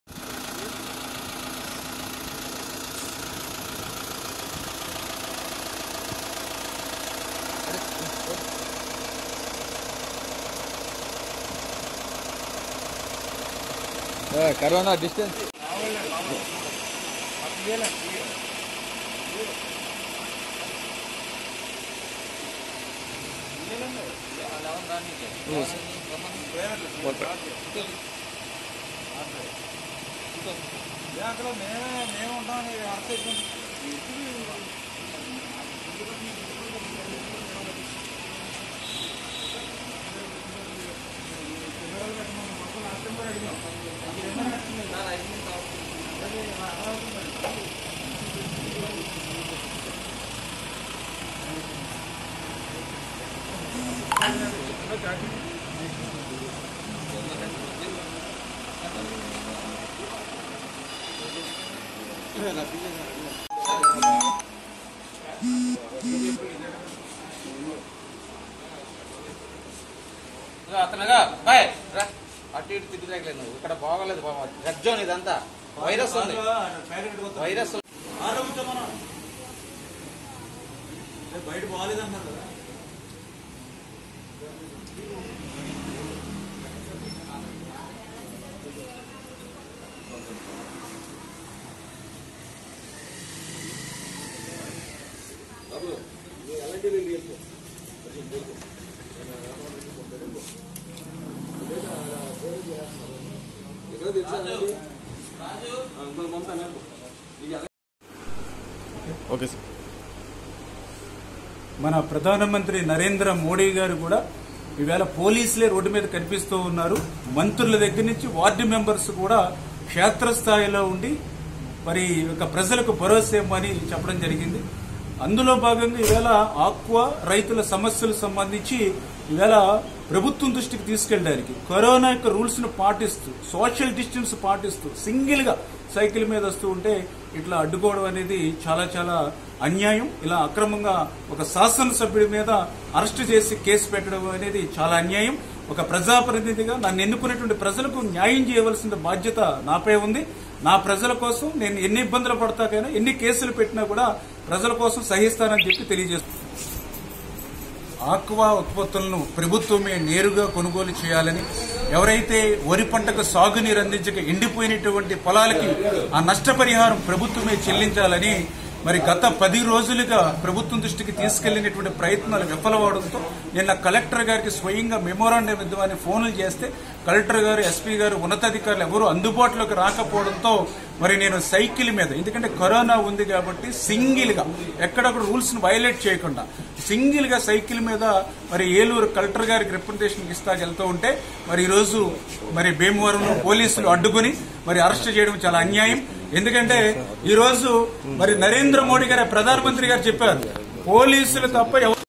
Palakollu MLA Dr.Nimmala Ramanaidu Garu Arrested || TDP Demands Justice For AQUA Farmers
Palakollu MLA Dr.Nimmala Ramanaidu Garu Arrested.